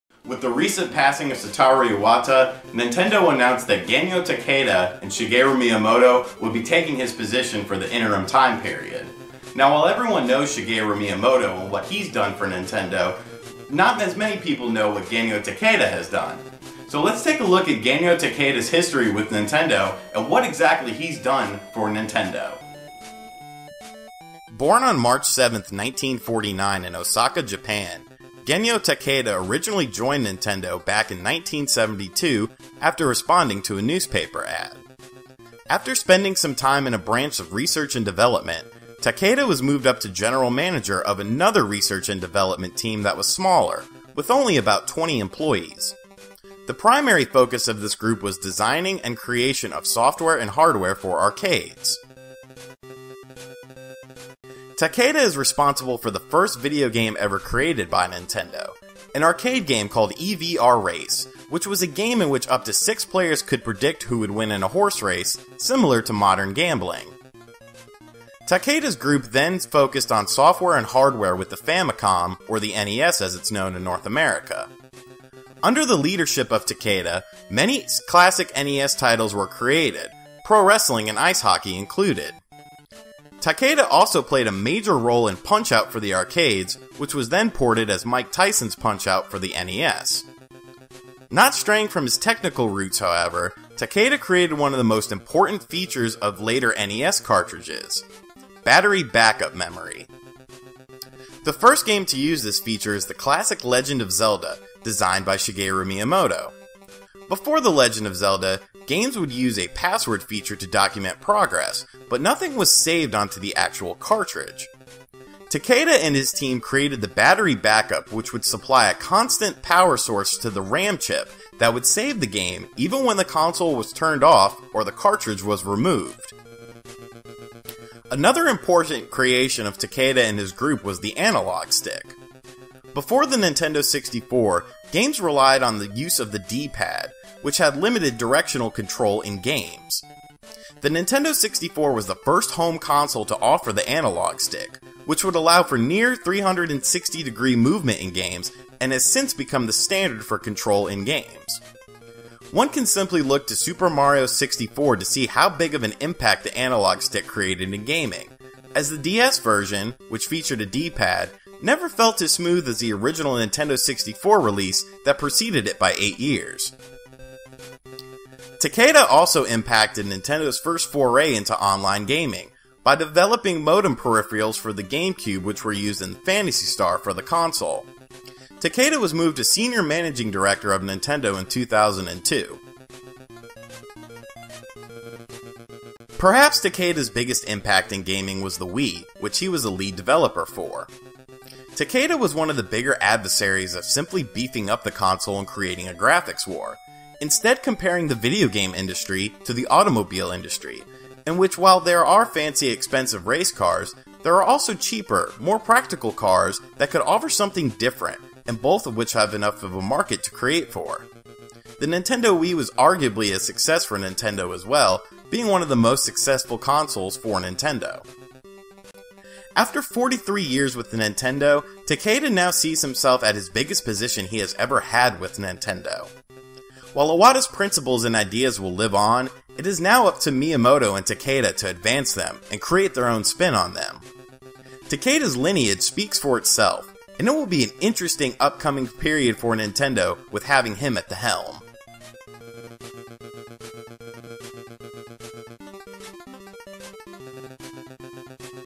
Genyo Takeda - Nintendo's Unknown Legend
With Mr. Iwata's recent passing, Shigeru Miyamoto and Genyo Takeda were announced to replace him for the time being. While everyone knows Mr. Miyamoto, just who is Genyo Takeda and what has he done? Let's find out.